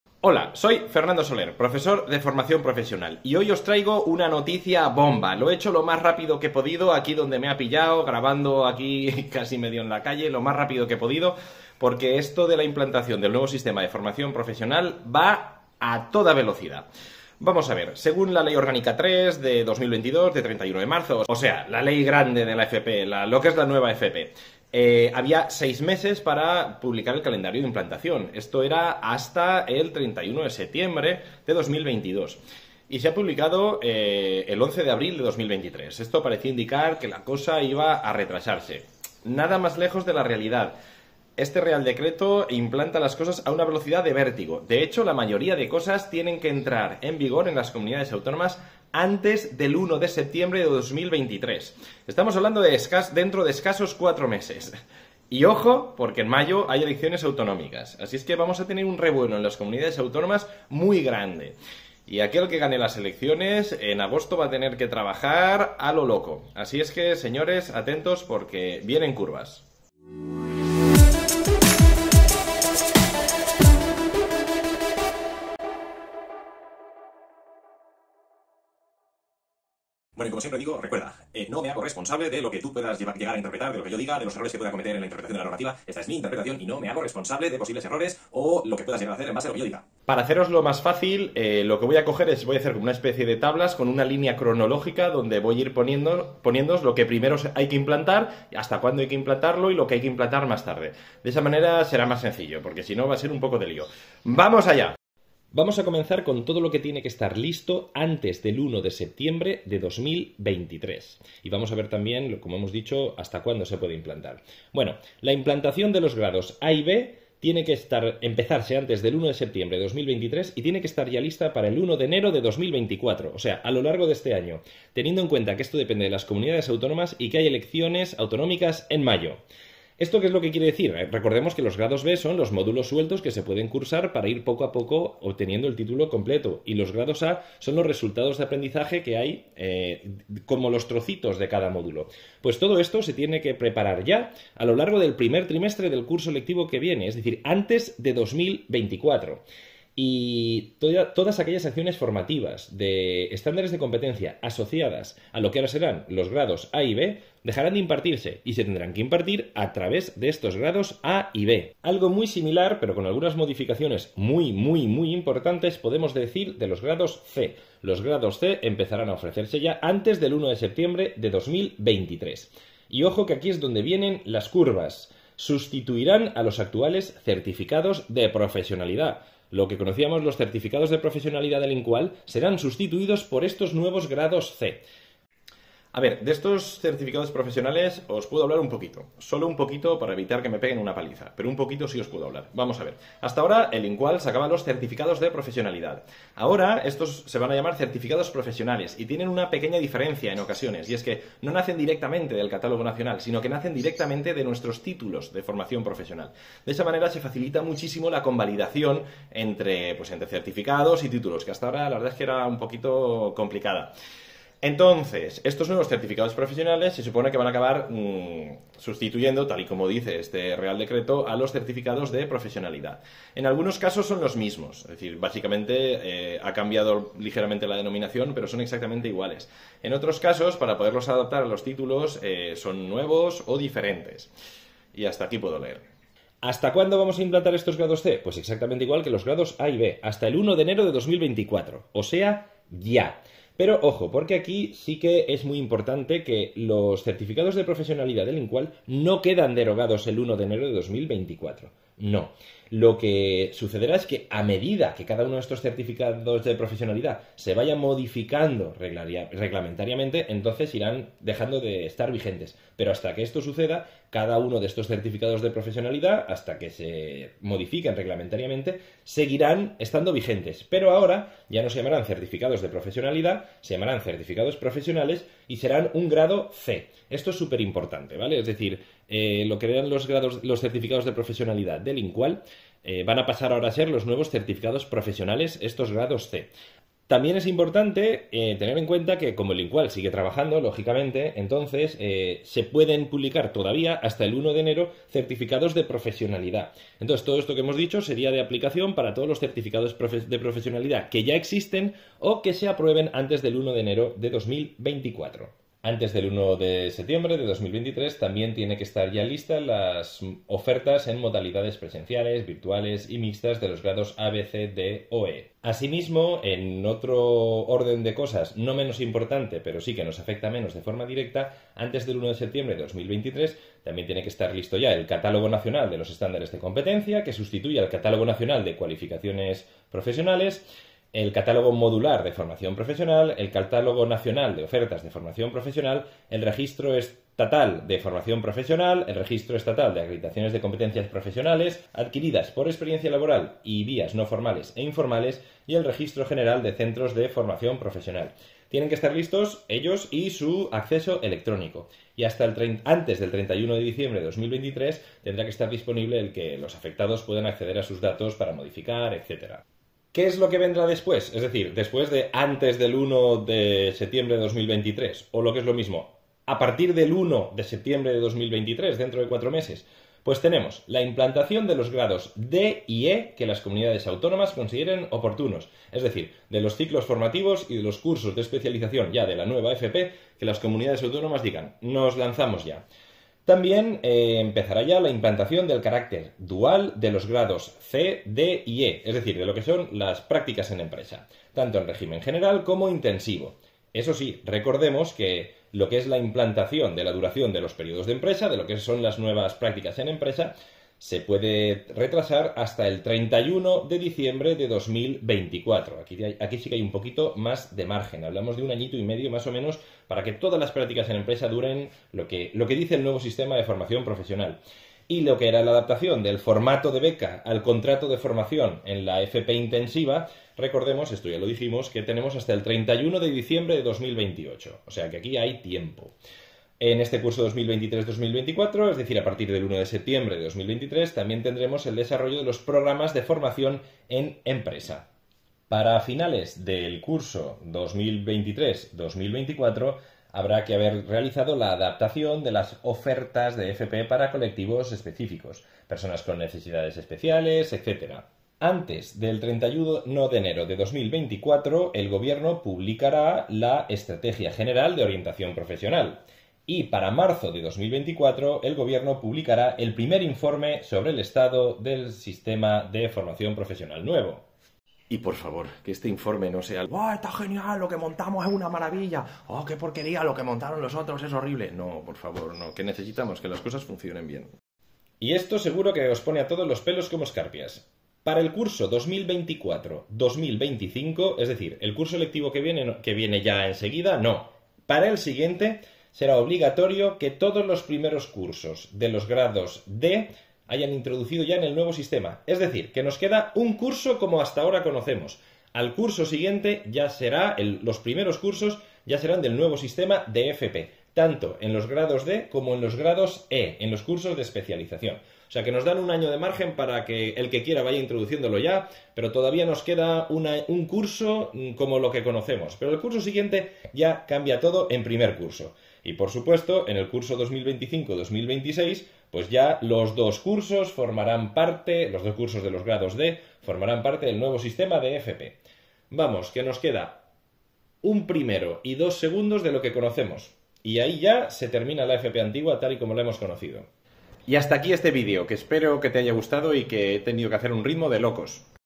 EXTRA, EXTRA!!! CALENDARIO IMPLANTACIÓN NUEVA FP!!!!!!
En este video estudiaremos el RD 278/2023 de 11 de abril, en el que se nos cuenta cuál será el calendario de implantación de la nueva FP.